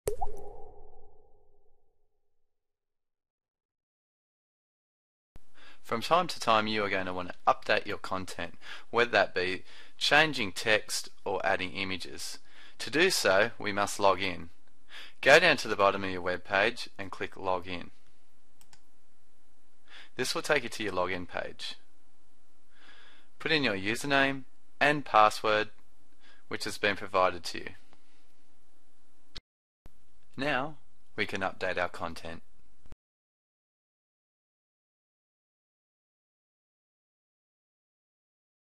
How to Login
Here is a simple video on how to login to your First Choice Media website.